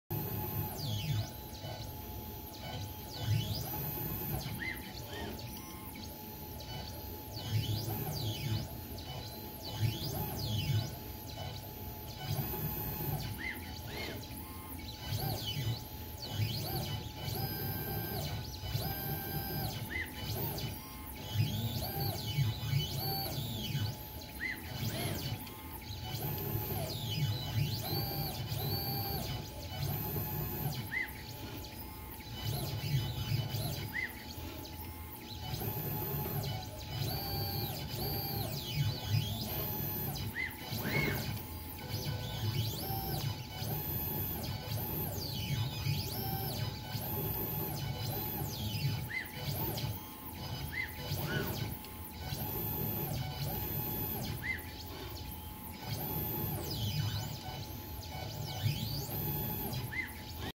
Onefinity CNC: Signs of Life
Just got it mostly assembled and doing a preliminary test drive.